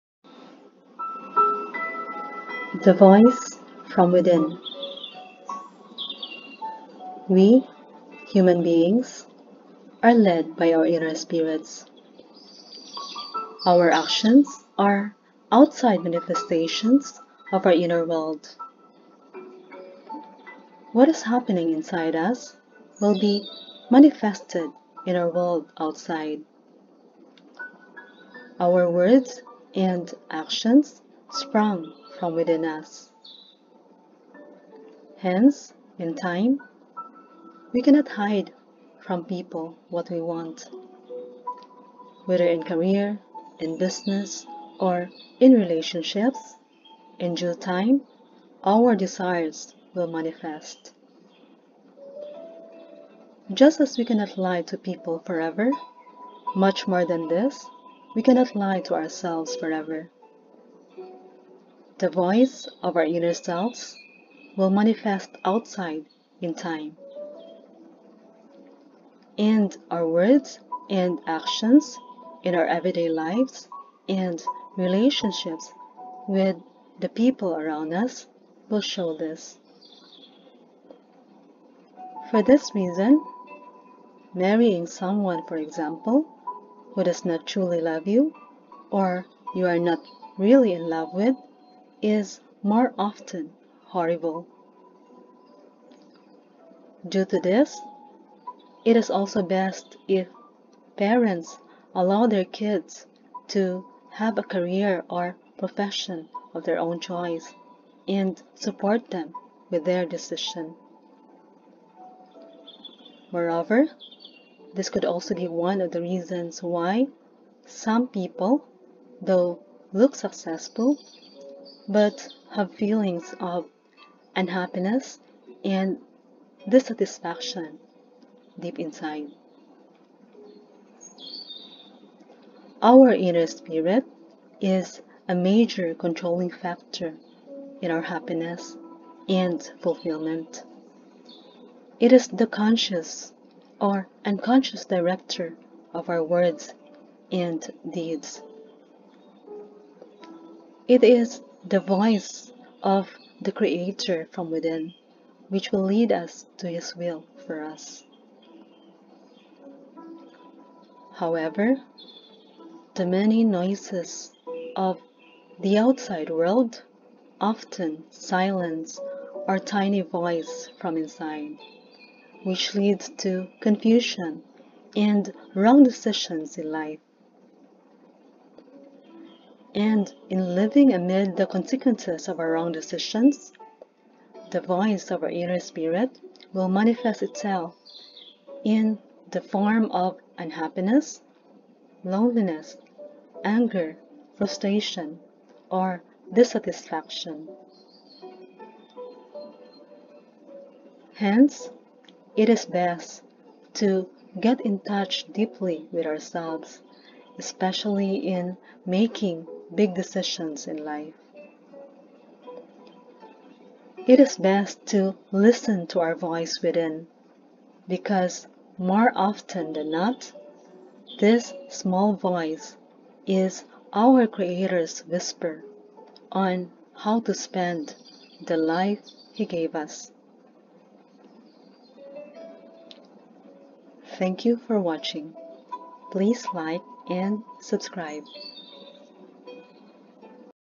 The Voice from Within / Life Calling
This video imparts my thoughts on the importance of listening to our inner voice, especially in making major life decisions.